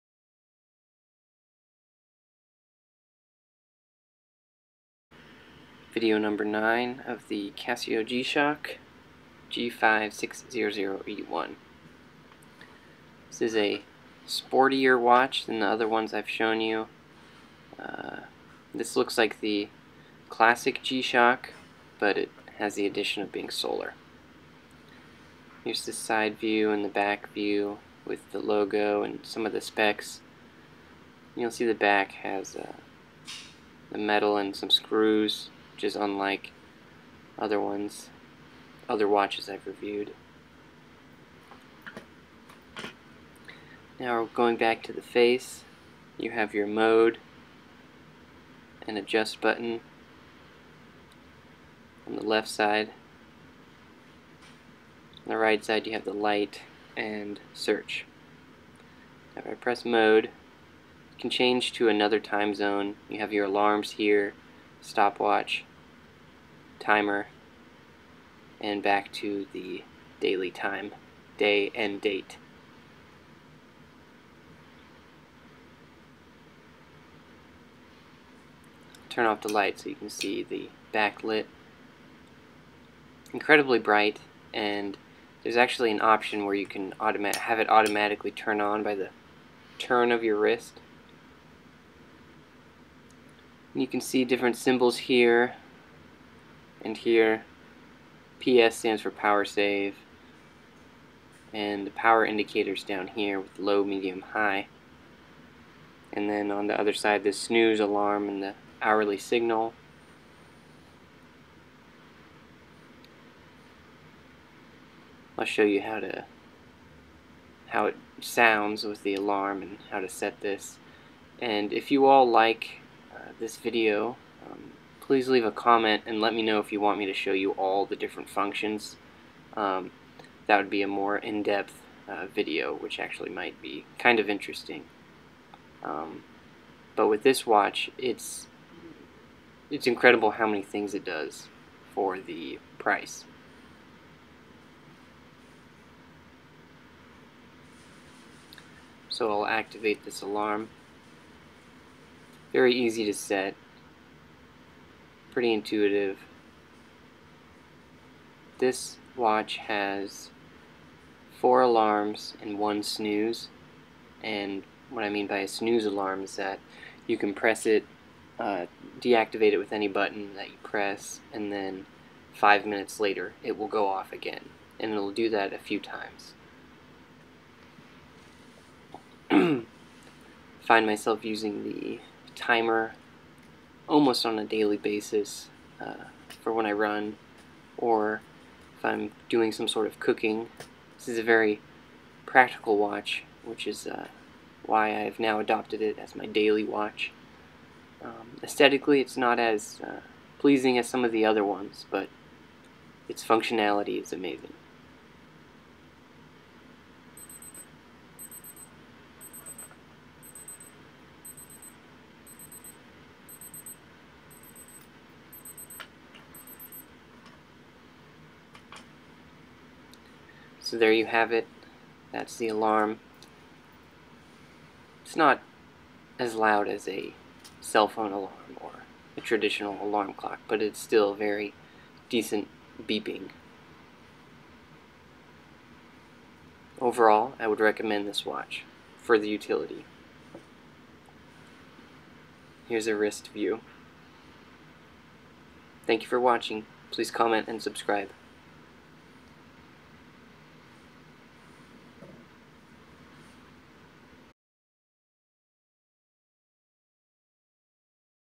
HD video #9 Casio G Shock G5600e 1
Casio G-shock Tough solar
Model:   G5600e-1
Case: Black  Resin
Movement:  Tough Solar
Features: Chronograph,
Day/date, world time,
count down timer, alarms,
shock protection, power save,
 power indicator,  backlight...
WR:  200m
Dimensions:46.7x43.2x12.7mm